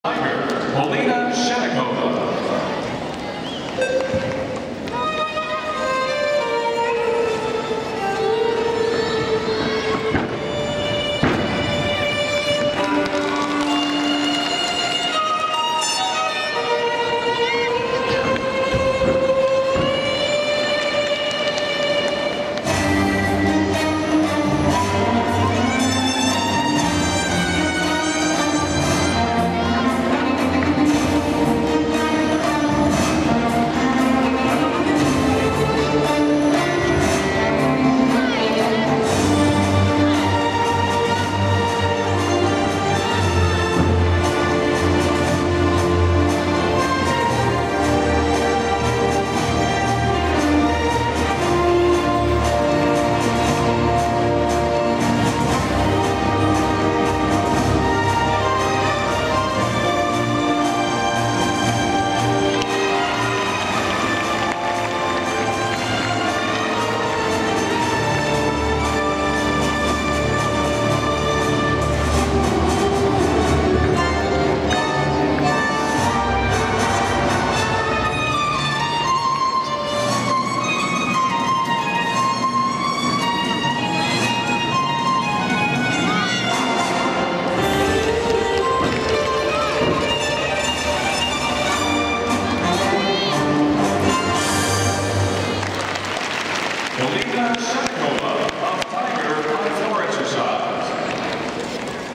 Polina Shchennikova - Floor Exercise - 2013 Secret U.S. Classic
Score: 13.650
July 27, 2013 - Sears Centre Arena - Hoffman Estates, Ill.